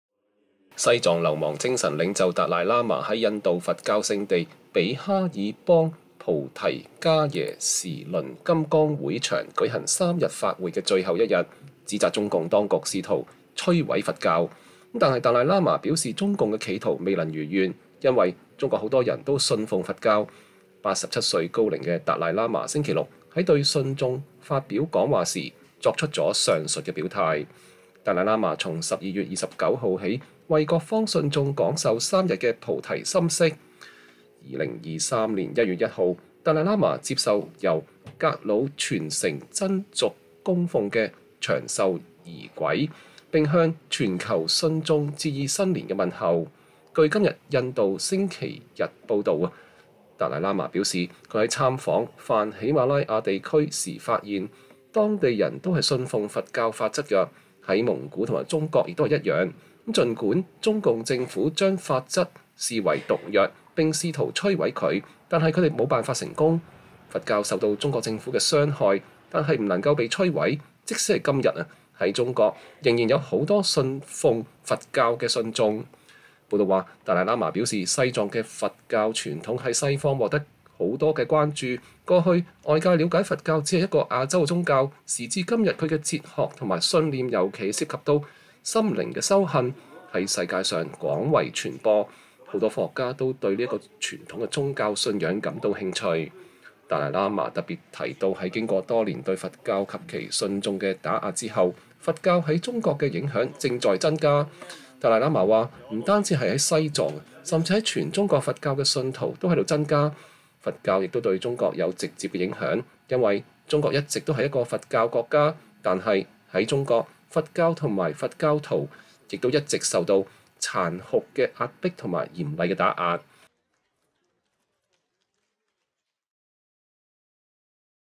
達賴喇嘛:中共試圖摧毀佛教但事與願違
西藏流亡精神領袖達賴喇嘛在印度佛教聖地比哈爾邦菩提伽耶時輪金剛會場舉行三天法會的最後一天，指責中共當局試圖摧毀佛教，但是達賴喇嘛表示中共的企圖未能如願，因為中國許多人都信奉佛教。 87歲高齡的達賴喇嘛星期六在對信眾發表講話時作出上述表態。